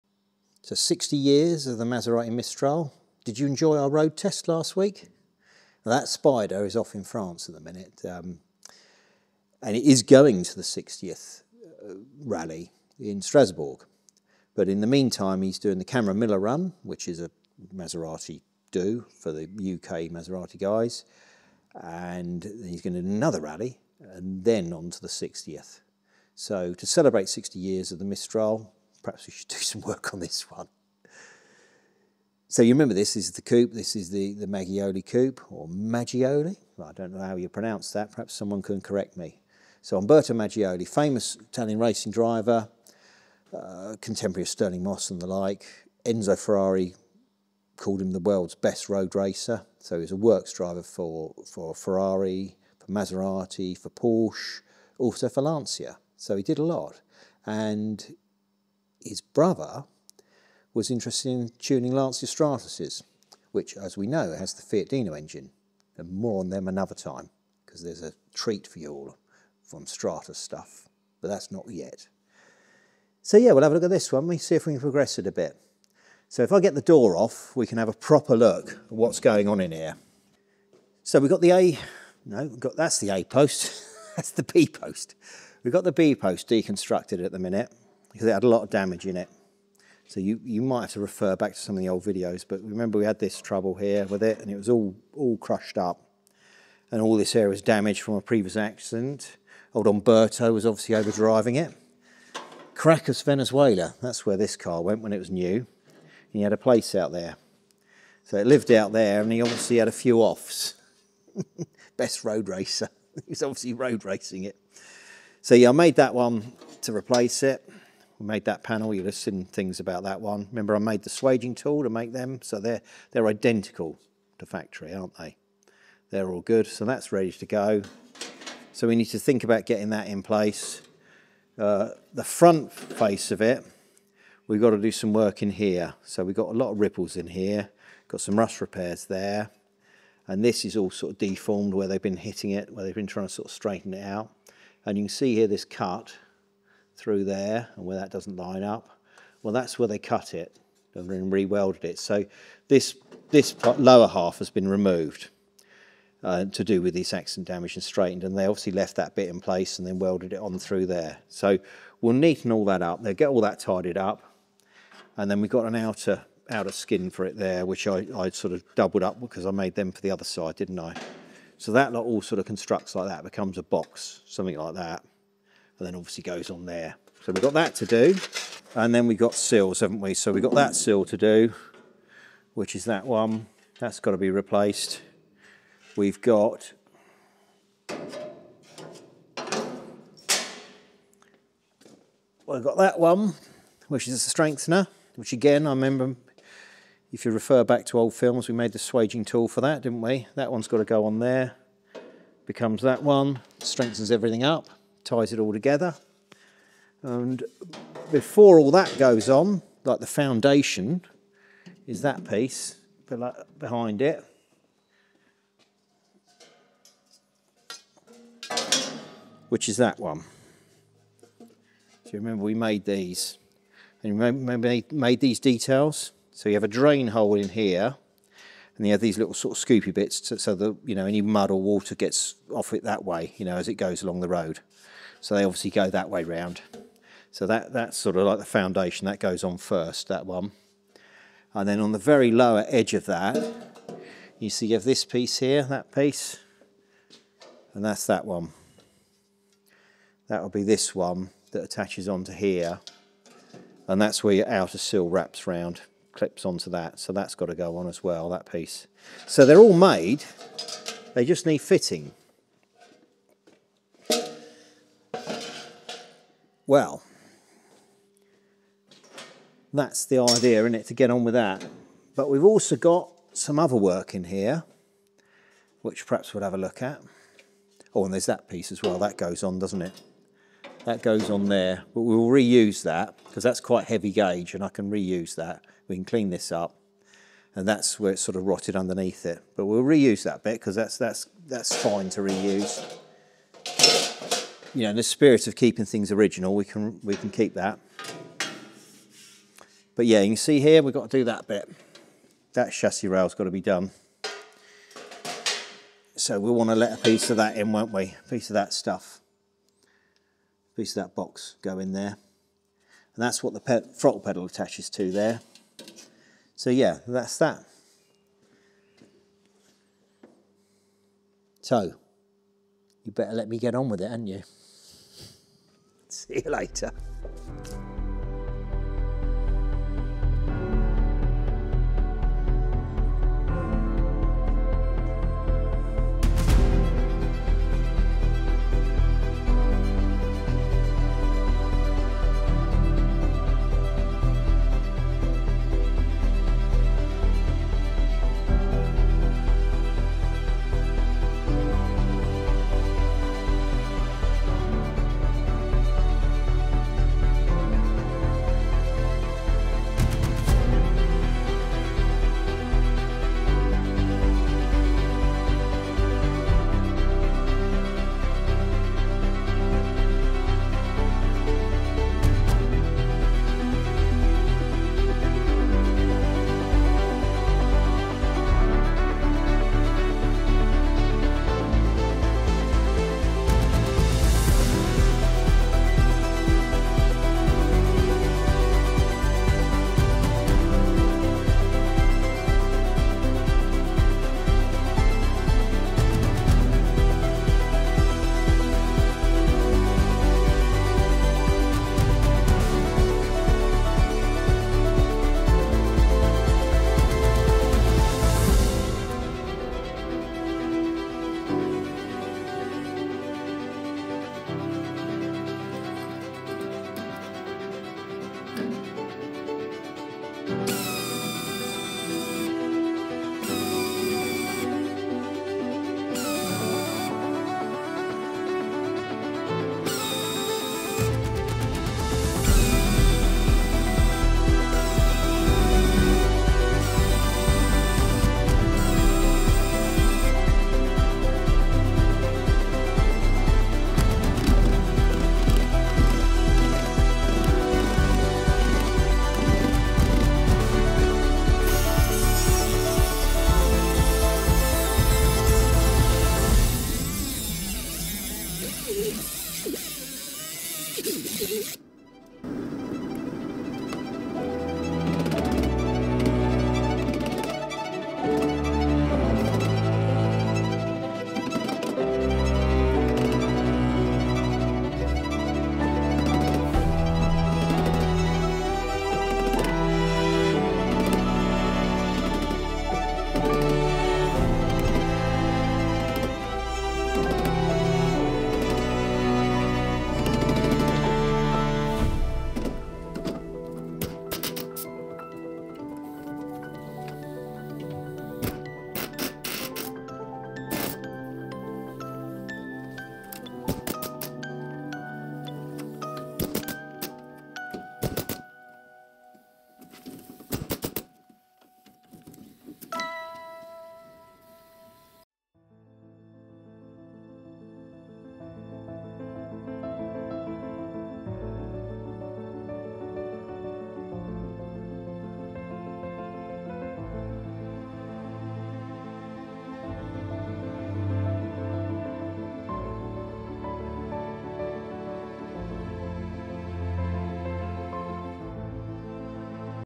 Maserati Mistral Restoration part 12.
The work continues with preparations for the replacement of the rocker panels and chassis outriggers.  This Maserati Mistral 4000 is an important car as its first owner was Umberto Maglioli  .  Who was a works driver for Ferrari, Lancia , Porsche and  Maserati !